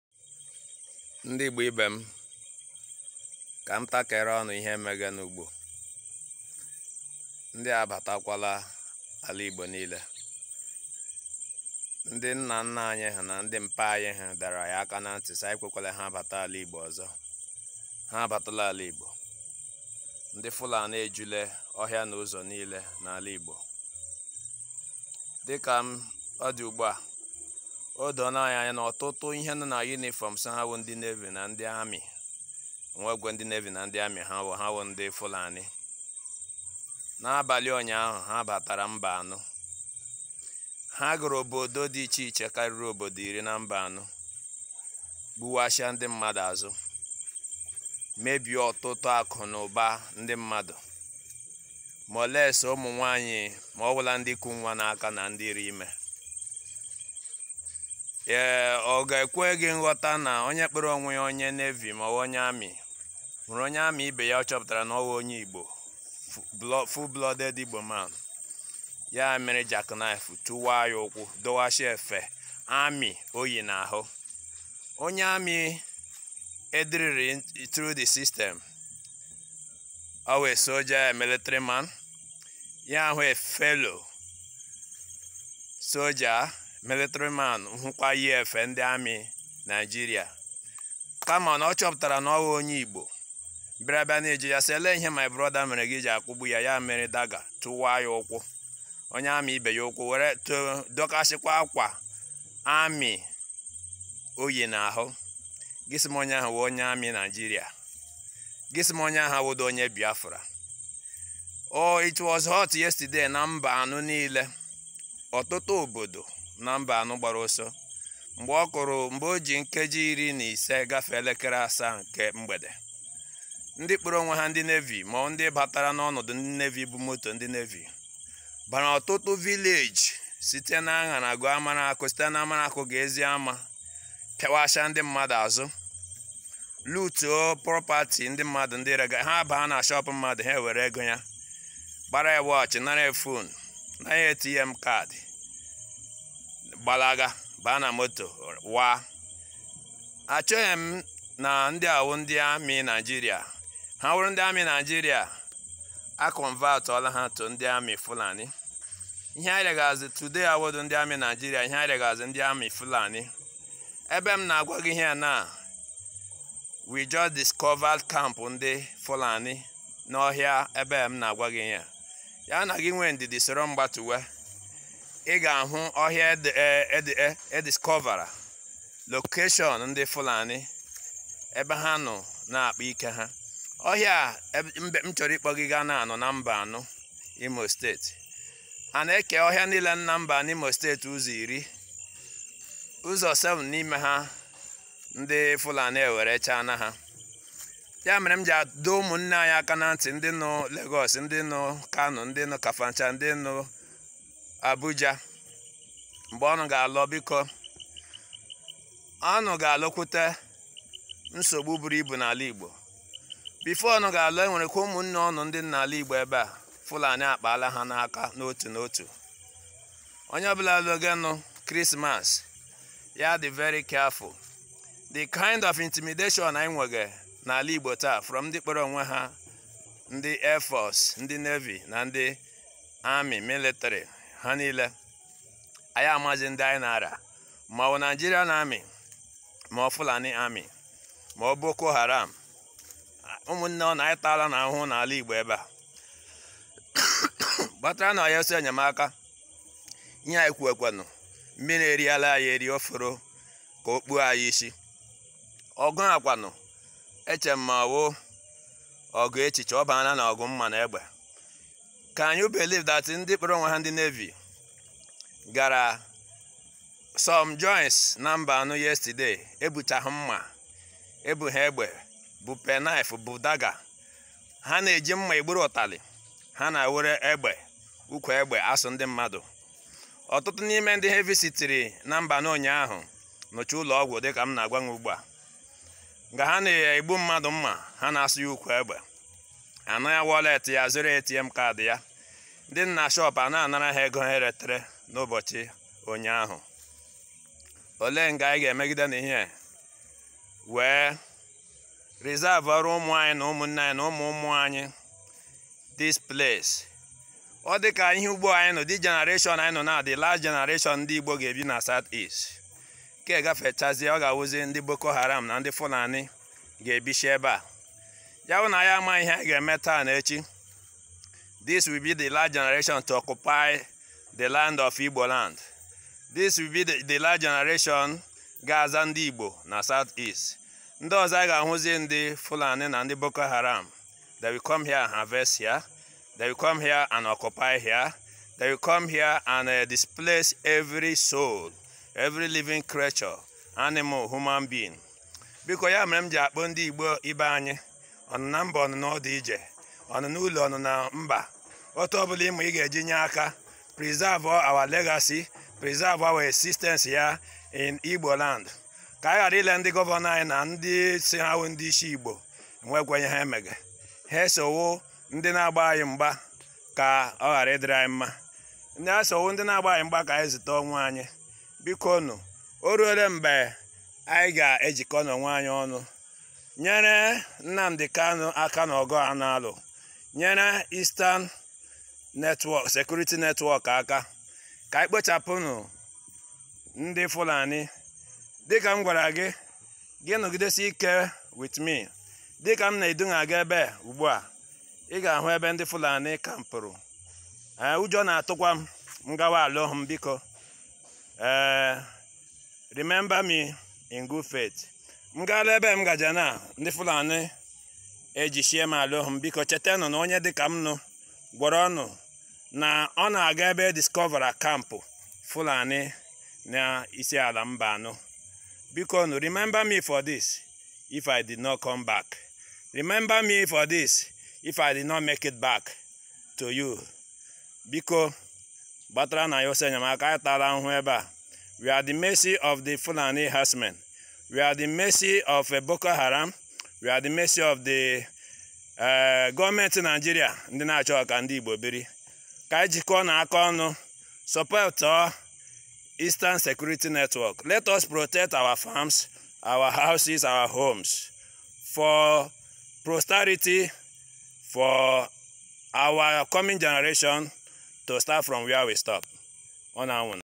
Listen to this IPOB member who survived 38 gun shots Fulani warriors.
this IPOB man was cornered by ten Fulani men who shot him severally but the bullets did not give him any scratches.